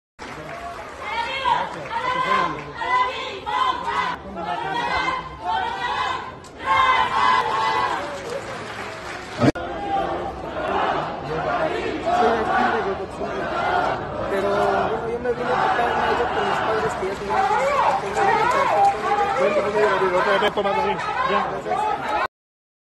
Sergio Salomón Céspedes se acercó a un hombre con discapacidad y que trabaja con un carrito adaptado
Durante el evento el gobernador Sergio Salomon Céspedes se acercó a Gabriel Martínez, un hombre que vive con discapacidad y que trabaja con un carrito adaptado.